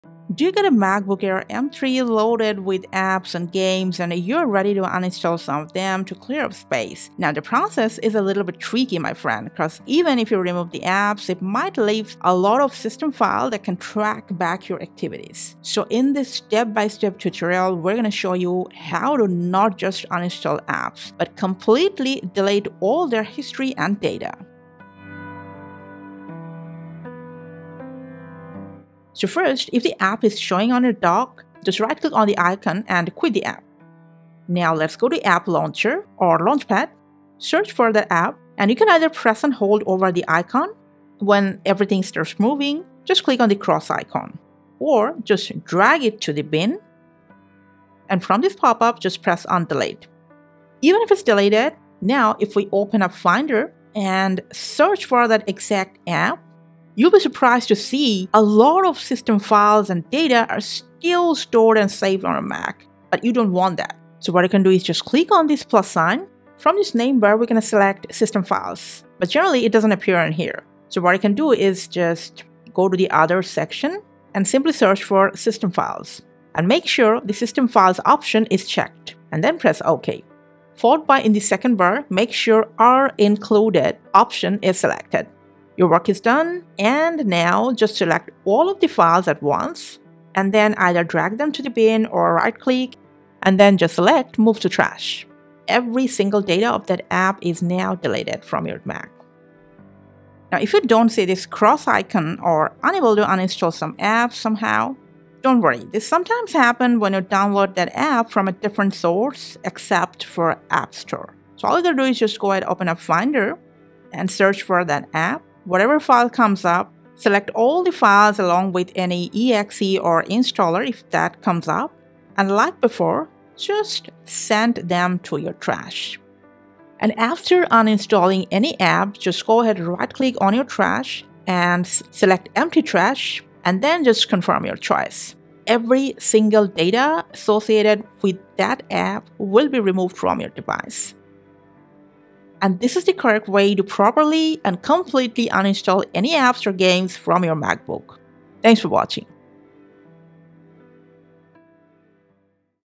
How to Uninstall Apps Permanently on MacBook Air M3! [MacOS Sequoia]
Need help to Delete Apps Permanently on MacBook Air M3 running on MacOS Sequoia as a new user? Can’t uninstall some 3rd-party apps on your M3 MacBook or want to fully delete any apps, including completely removing all data and history of the app from your Mac? The Fix369 Team has your back.

Simply follow the 2-step guide to permanently uninstall any programs or Apps from MacBook Air with Apple Silicon M3 Chip running on the latest iOS effortlessly. If you can’t delete some apps on your M3 Mac, we will explain why it is and how to delete them completely as well.

0:00 Intro: Uninstall Apps Permanently on MacBook Air M3
0:27 Step 1: Delete the Unwanted Apps from MacBook M3
0:52 Step 2: Completely Uninstall any Apps from MacBook M3
1:41 Why can't I uninstall some Apps, and how to delete them